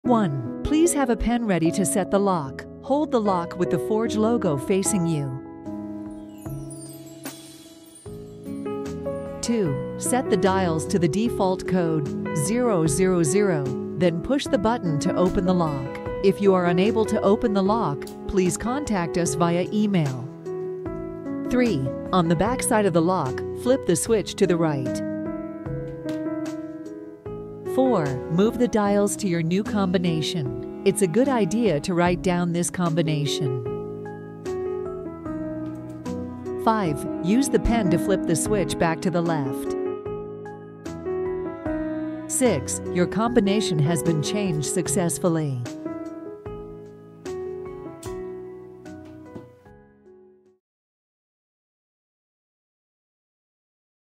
How to change your combination on Forge 21105 model TSA Lock.
A short video guides you through the step-by-step process of changing the code on your Forge TSA Approved Luggage Lock.